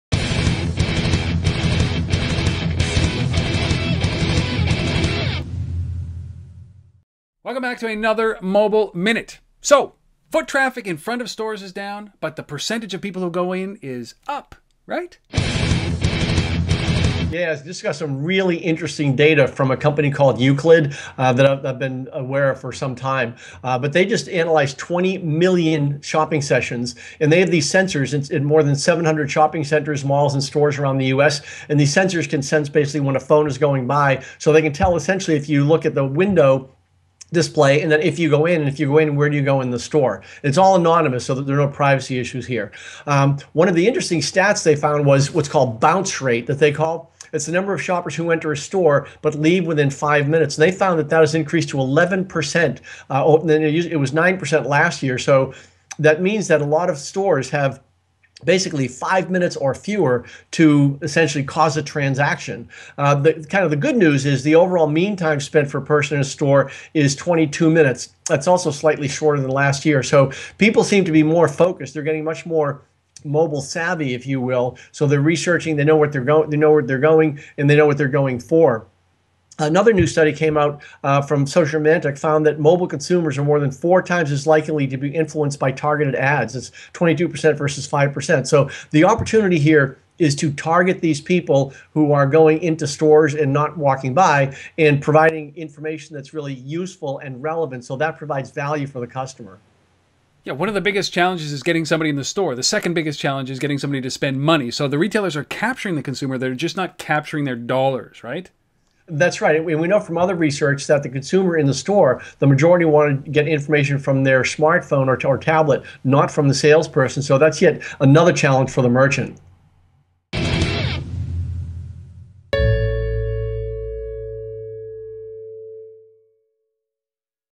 Mobile Minute #209: The pinpointed mobile consumer
Two recent studies show us the change in the shopping patterns for many consumers that carry a smartphone. These studies reveal two very important trends for retailers and what influences the consumer to enter and buy from a business.